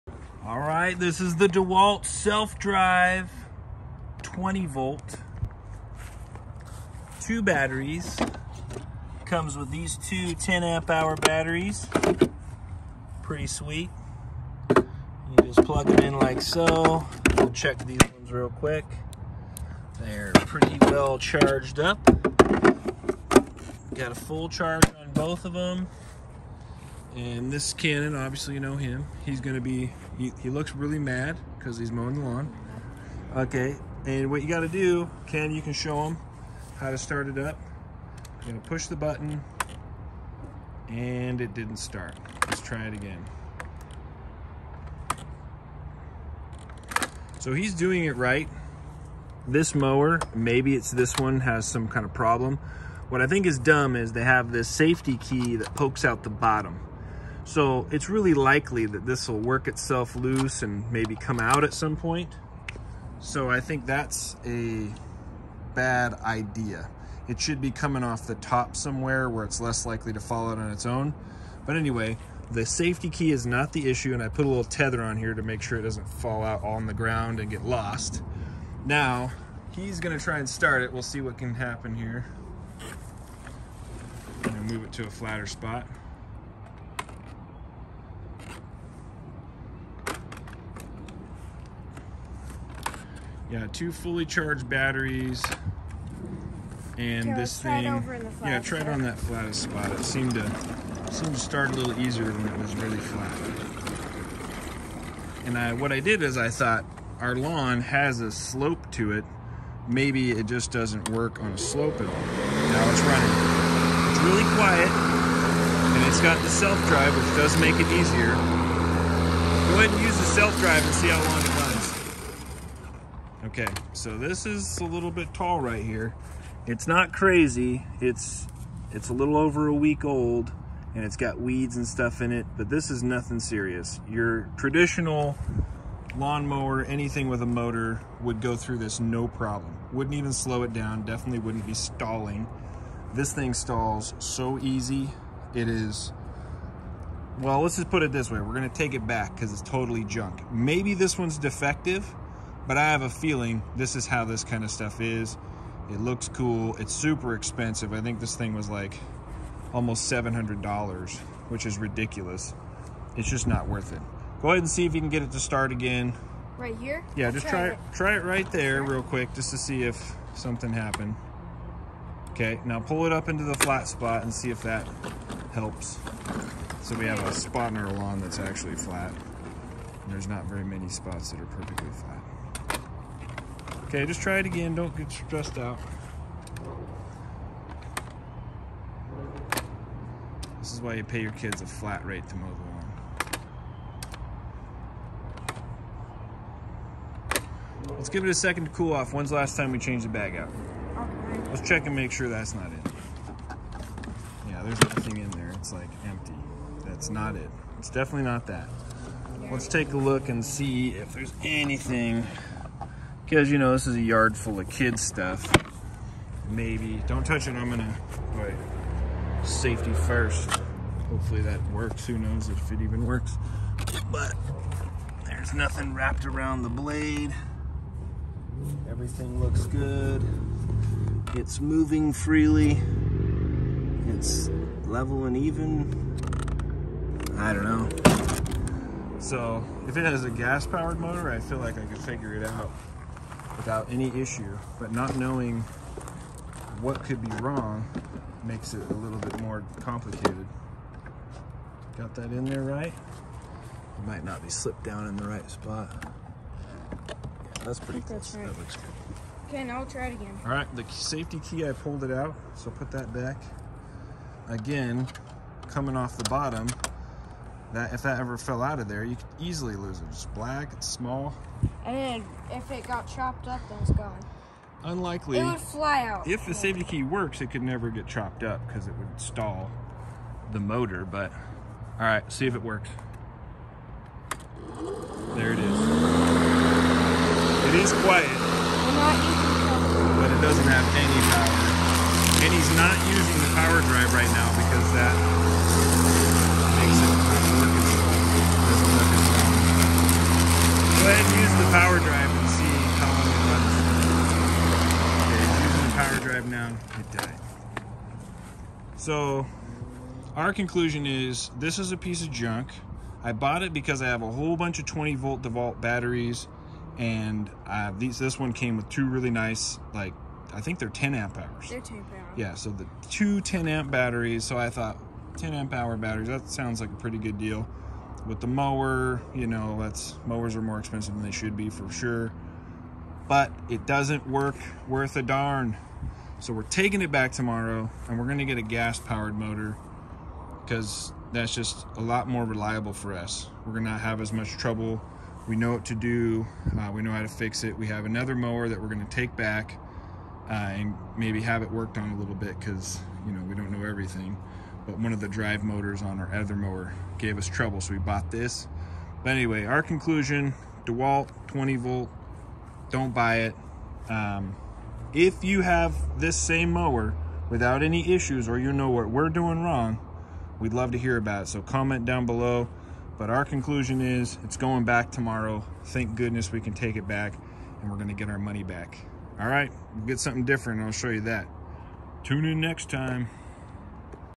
Dewalt 20v Self Drive Brushless Mower-Treasure or Trash?
Our lawn is small, the grass is dry, this tool is trash